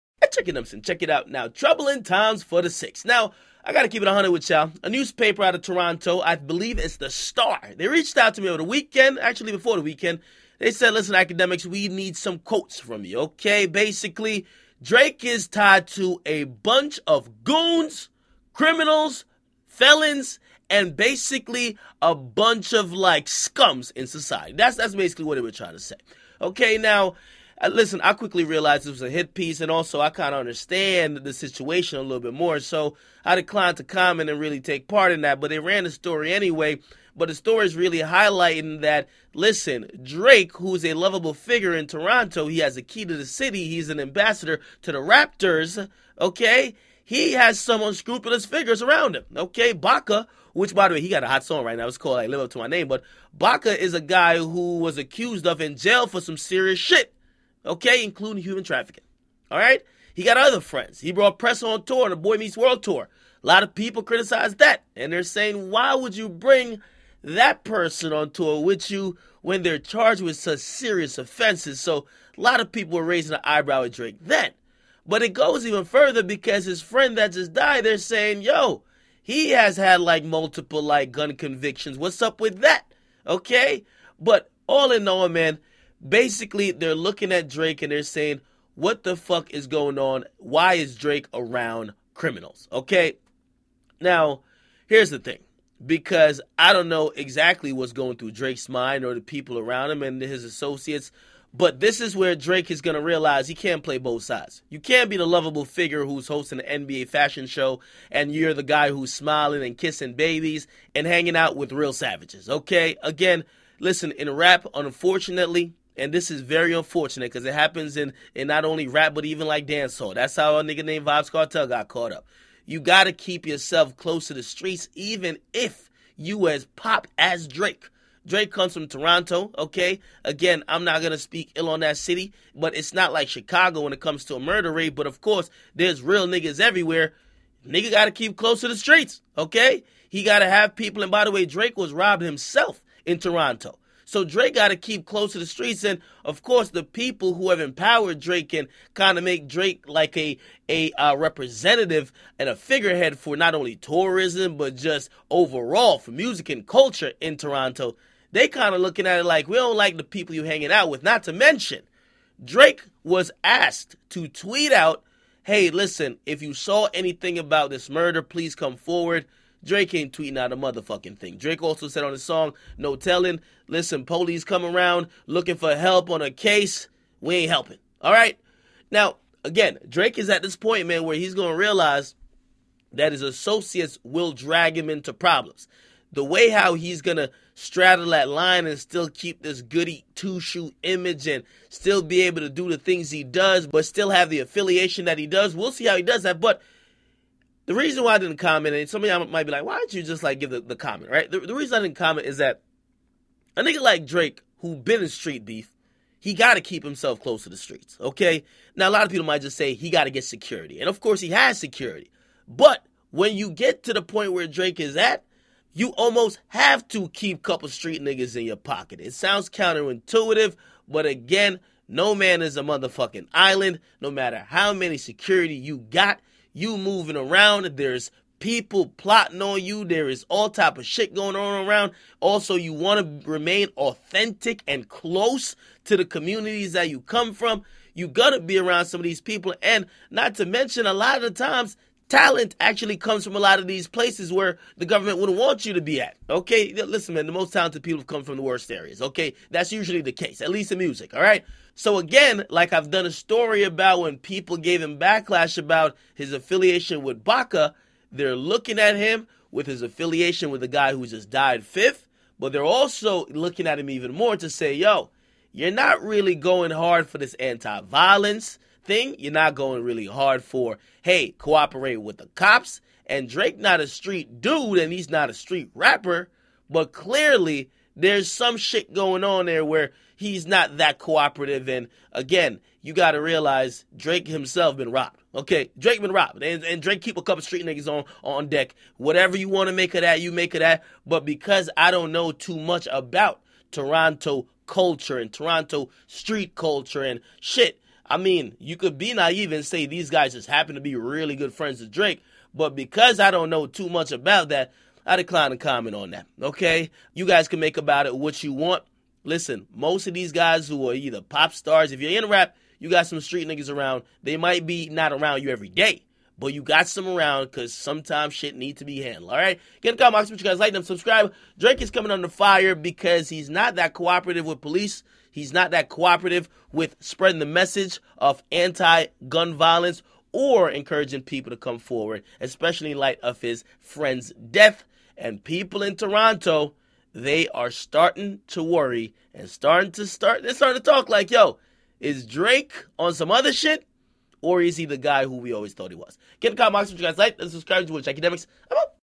Police have questions about Drake hanging w/ Criminals after his Friend 'FIF' gets gunned down.
DJ Akademiks Speaks on Police having questions about Drake hanging w/ Criminals after his Friend 'FIF' gets gunned down.